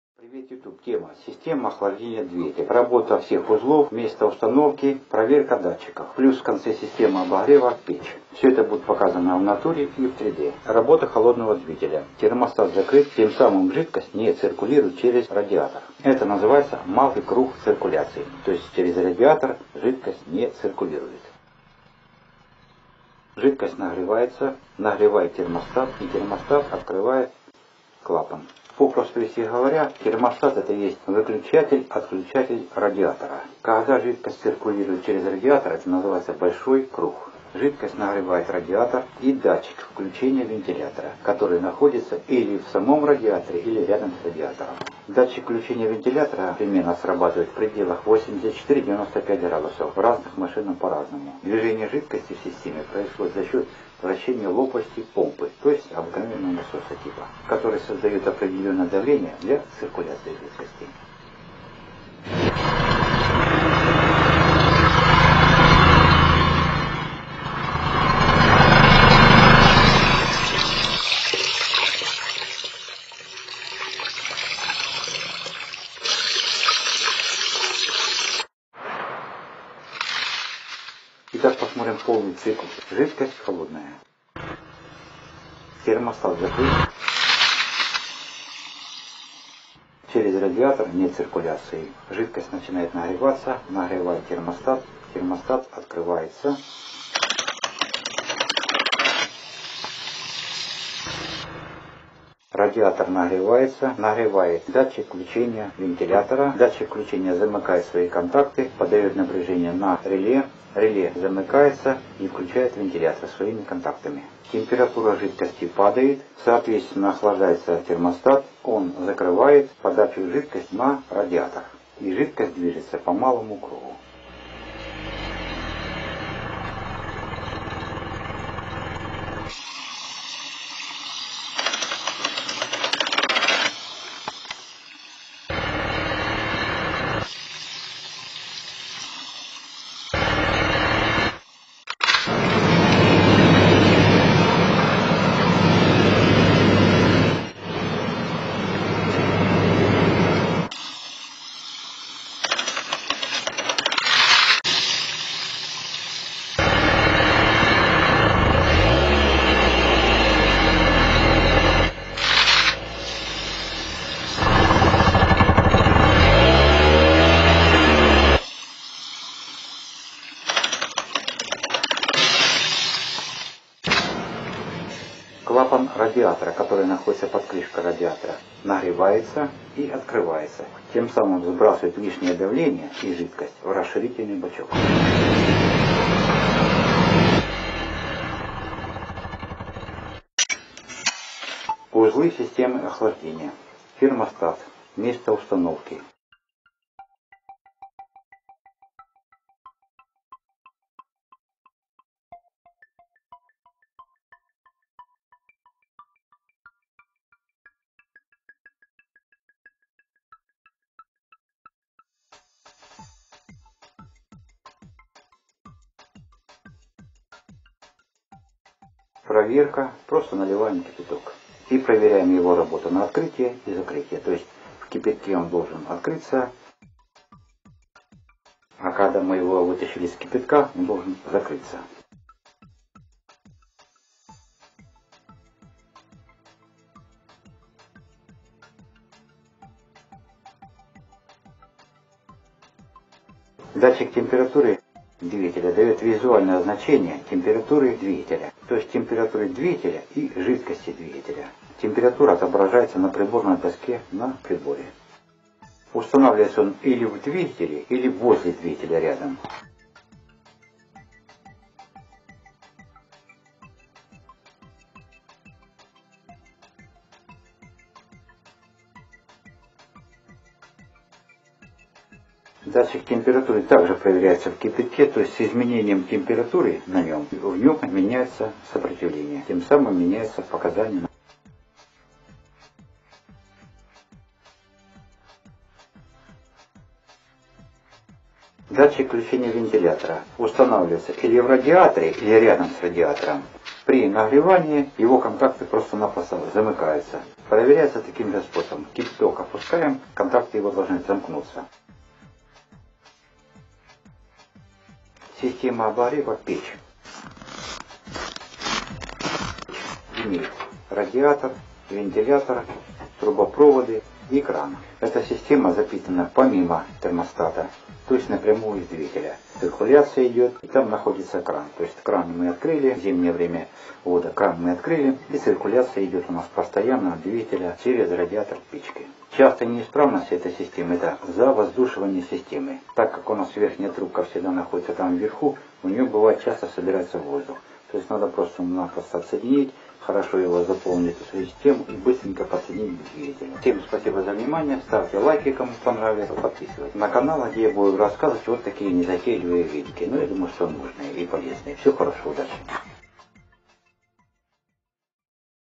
✅🚓🚛Системы охлаждения.3D.Car. Cooling system 3D.
Система охлаждения авто.Работа узлов,датчиков их проверка,места установки .Обогрев салона печь,нюансы..видео в зD  и натура полный процесс движения жидкости и работы всех узлов..